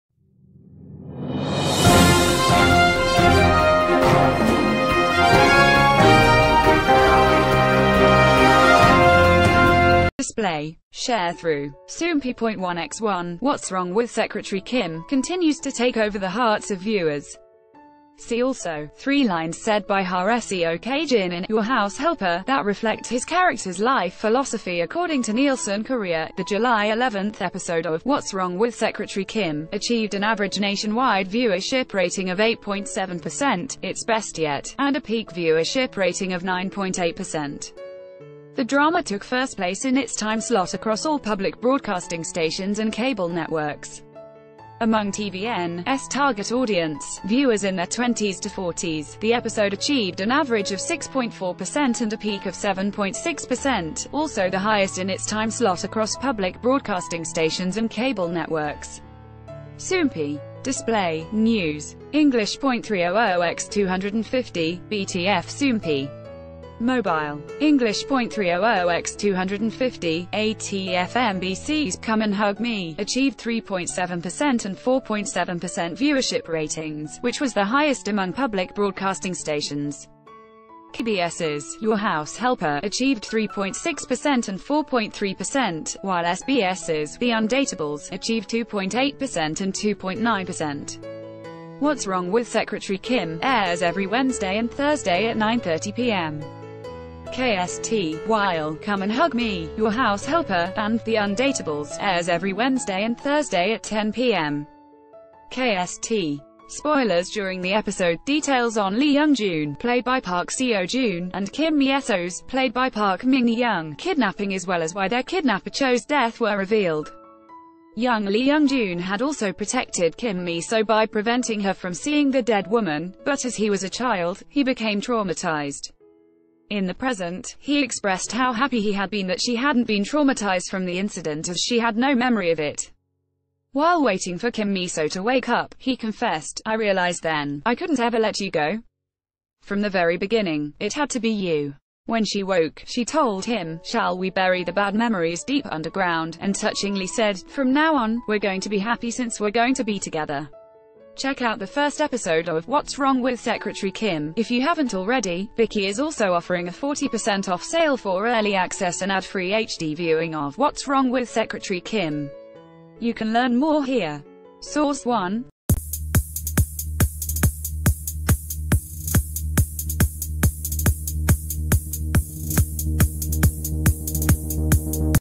"What's Wrong With Secretary Kim" Dominates Over Wednesday-Thursday Dramas With New Personal Best...
"What's Wrong With Secretary Kim" Dominates Over Wednesday-Thursday Dramas With New Personal Best In Ratings
Display. Sharethrough. Soompi.1x1 “What’s Wrong with Secretary Kim” continues to take over the heart...